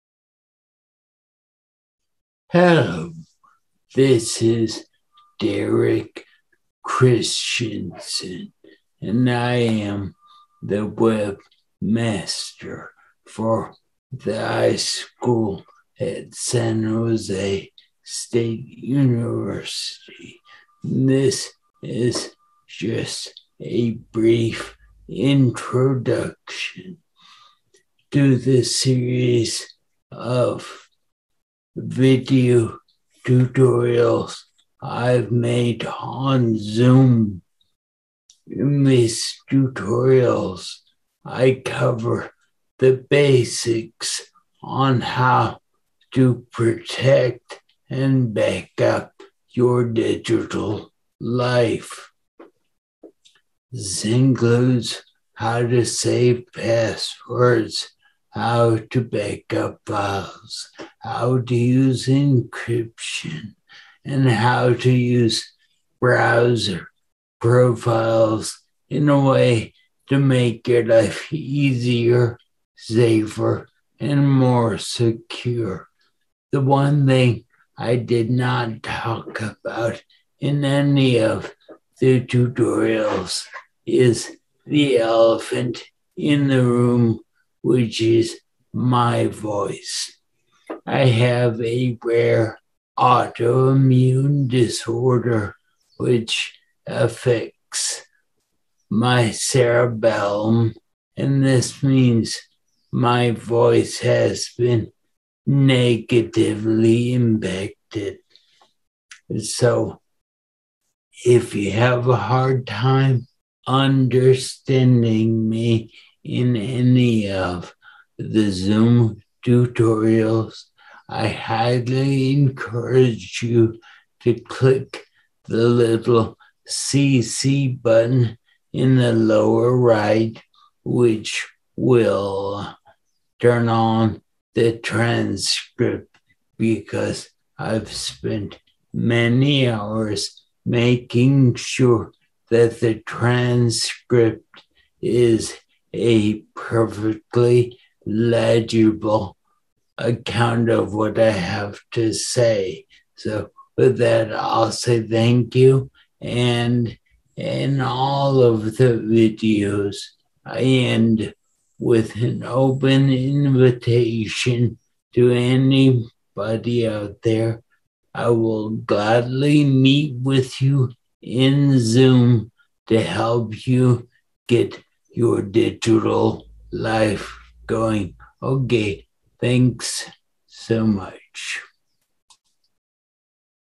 Data Backup: Introduction
Derek Christiansen introduces himself and explains purpose of tutorials.